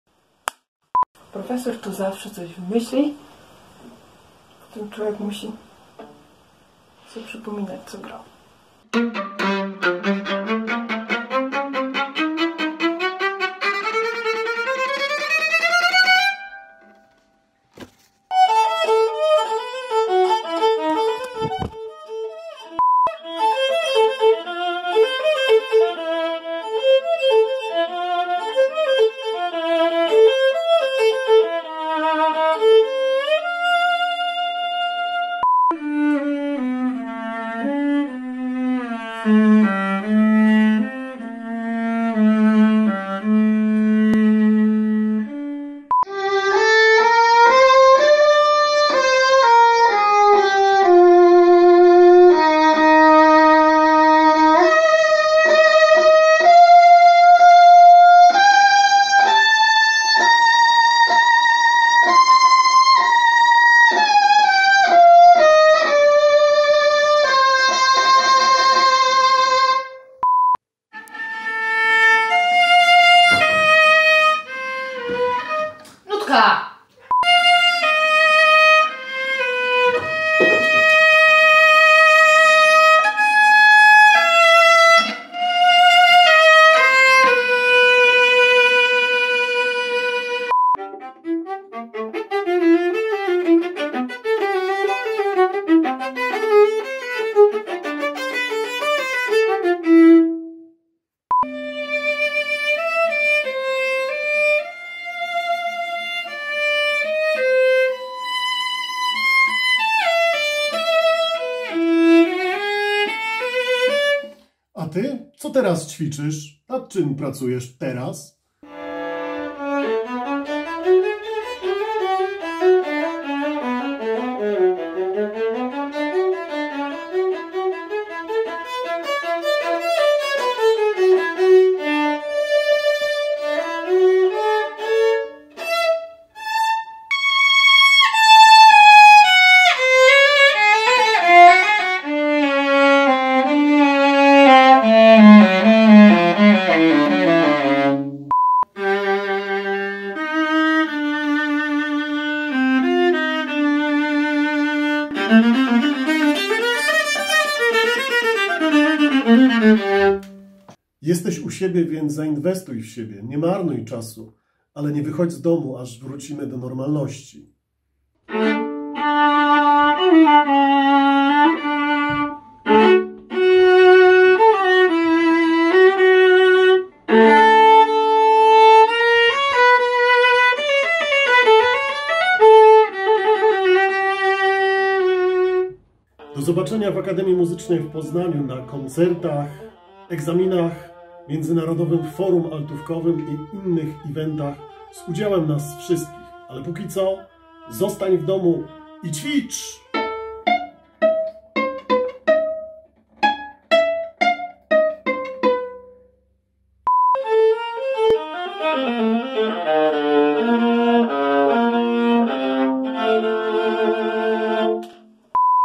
Quarantine viola practice 2020 wersja polska
W nagraniu udział wzięli altowioliści z klasy dr hab. Marcina Murawskiego, profesora nadzwyczajnego Akademii Muzycznej w Poznaniu: Aleksandra Ciapa, Marianna Kalicka, Antonina Kucharska, Michalina Matias, Aleksandra Ruciak, Anna Utmańczyk, Nancy Xie, Jakub Karczewski, Leik Shah Bukhari oraz gościnnie Maria Stugiewicz.
Stay home and practice!
Koncept: Marcin Murawski
Montaż: Tomasz Jarosz